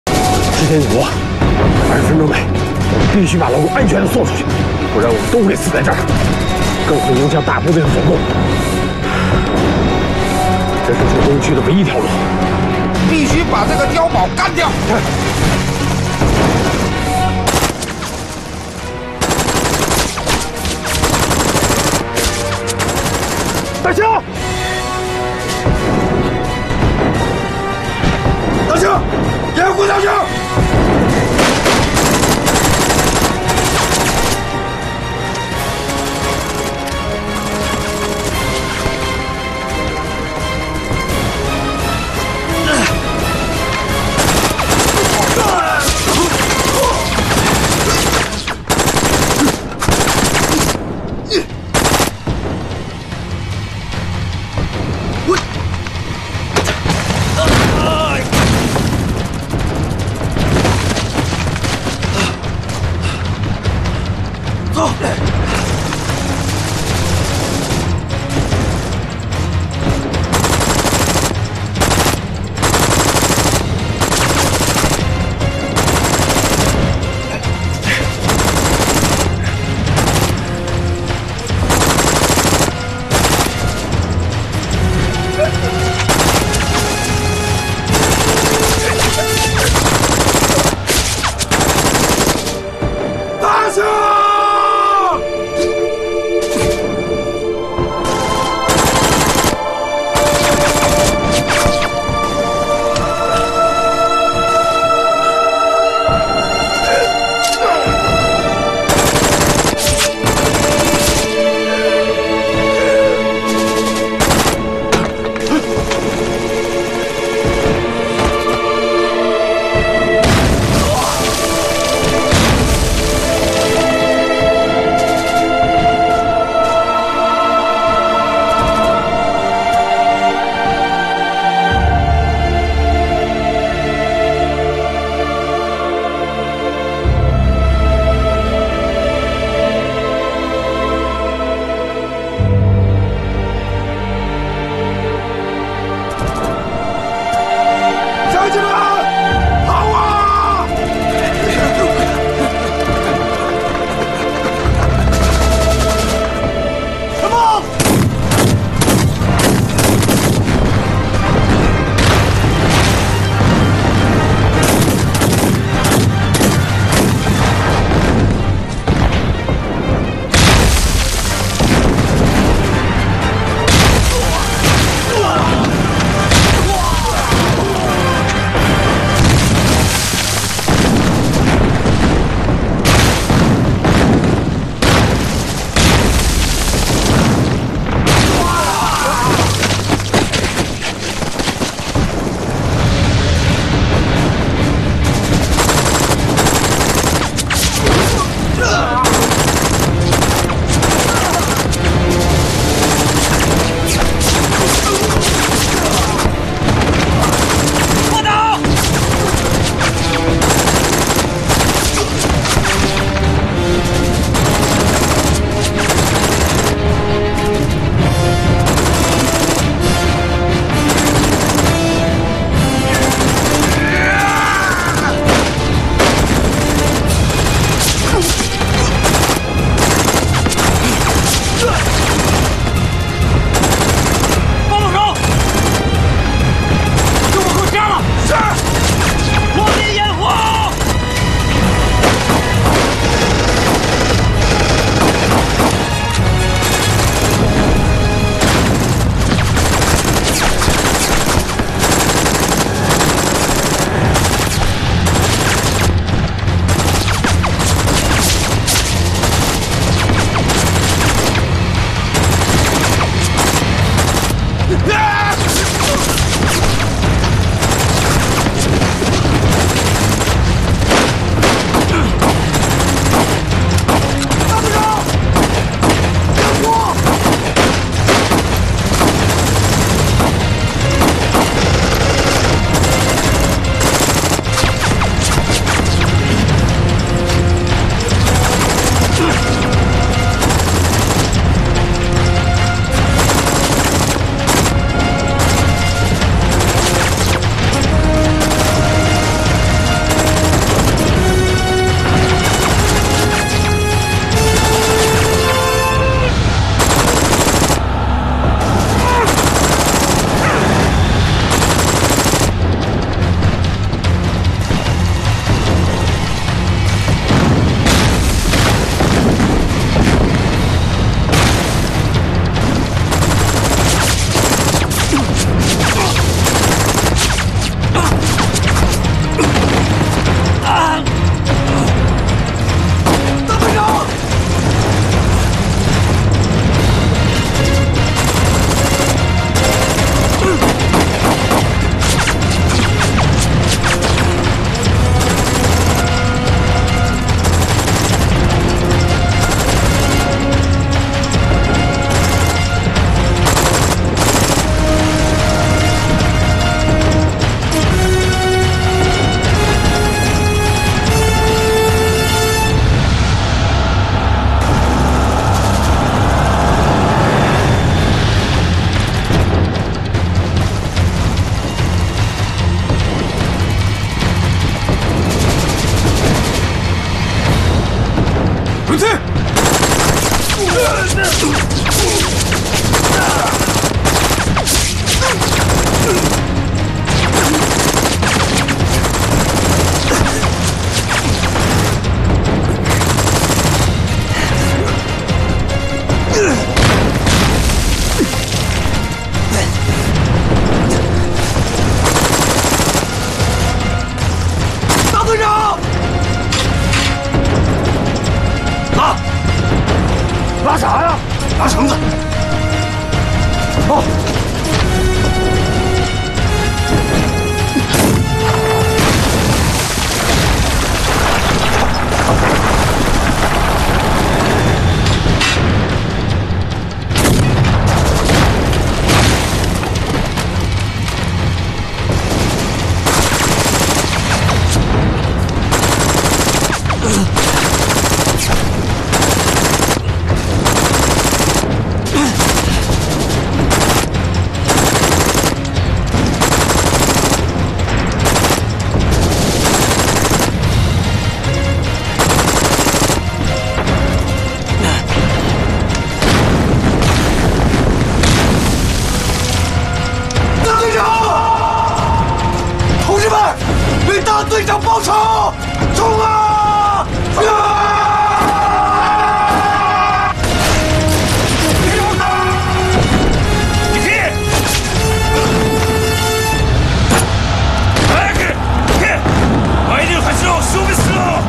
日軍碉堡火力強大，為了讓百姓安全撤離，共軍戰士用自己的肉體堵住了日軍槍口！⚔ 抗日
ls 40.1